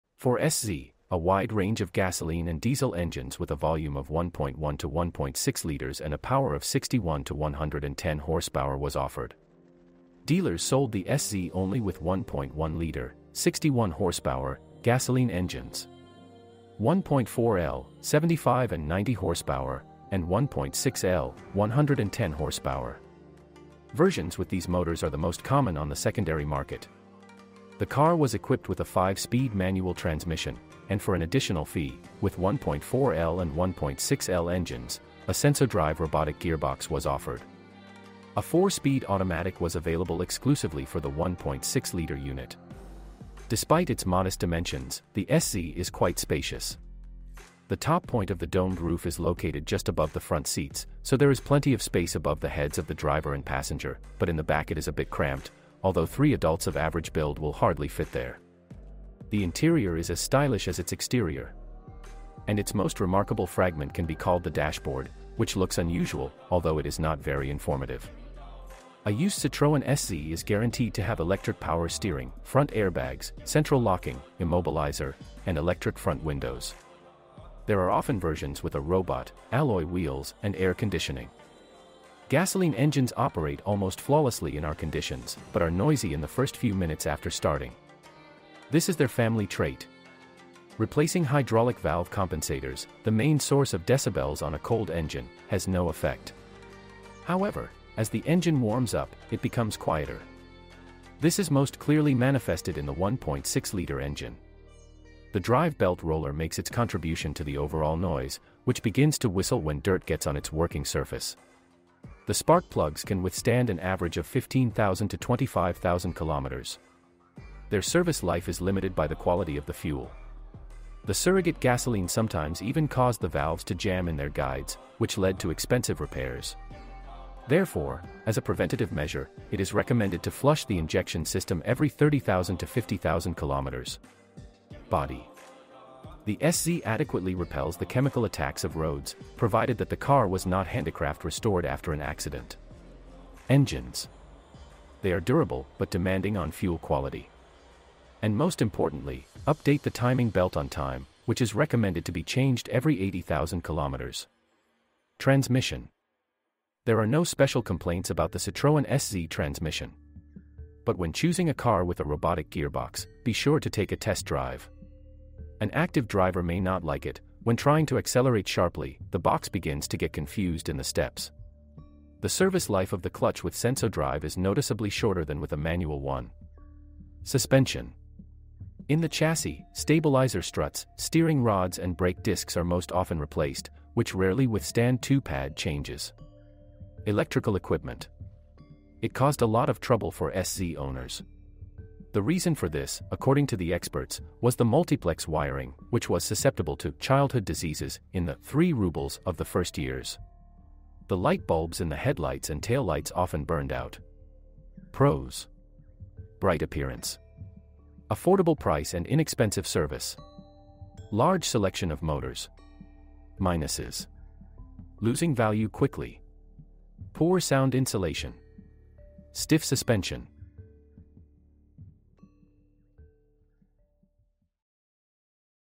TOP Things that will BREAK on your Citroen C3
How many miles is bad for a used Citroen C3 1? Are used C3 expensive to maintain?